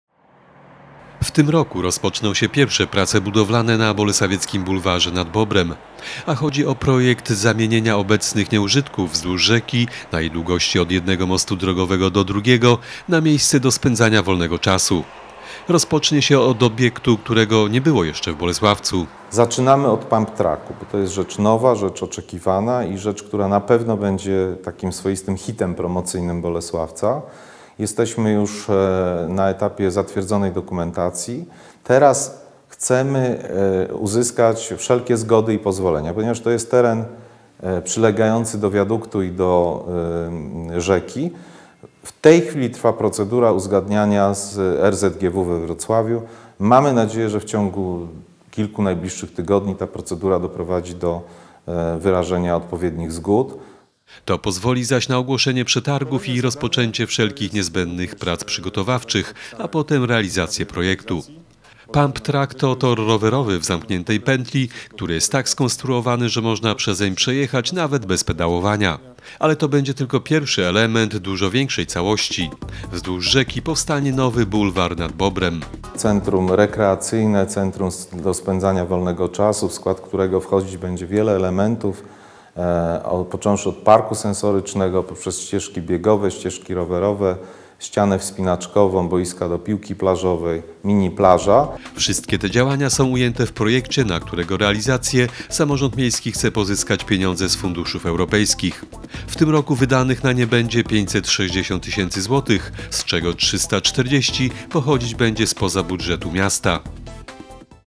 Pumptrack nad Bobrem
W tym roku wykonany ma być pierwszy etap budowy Bulwaru nad Bobrem czyli zagospodarowania miejskich terenów nadrzecznych pod funkcje sportowo-rekreacyjne. Pierwszym elementem Bulwaru będzie specjalny tor rowerowy: pumptrack.